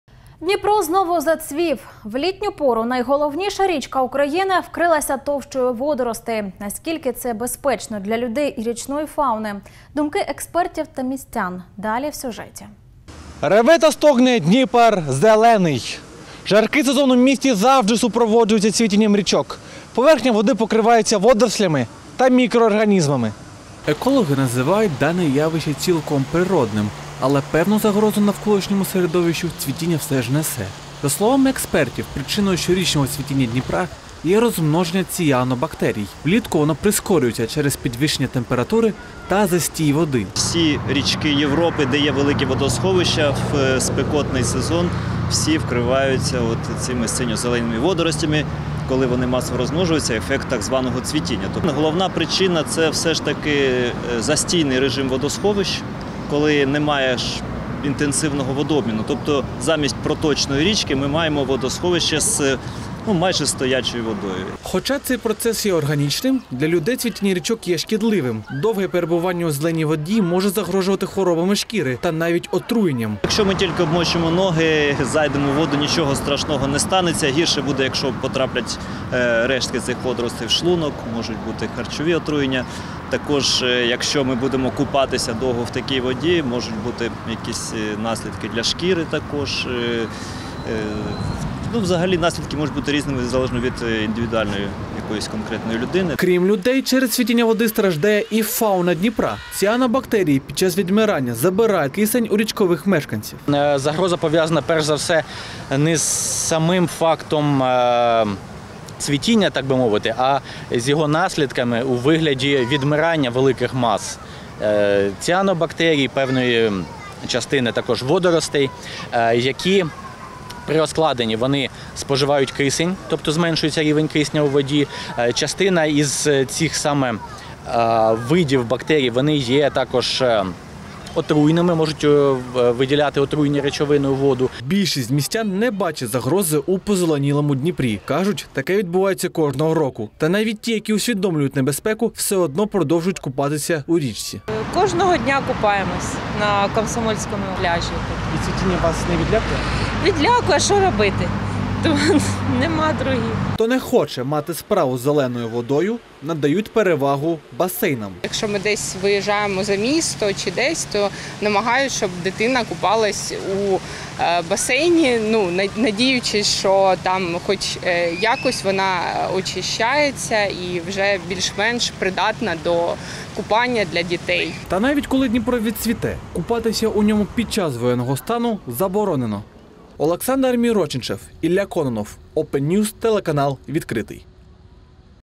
Річка Дніпро позеленіла - причини та потенційна небезпека цвітіння для дніпрян
Дніпро знову зацвів. В літню пору найголовніша річка України вкрилася товщею водоростей. Наскільки це безпечно для людей  і річної фауни. Думки експертів та містян – далі в сюжеті.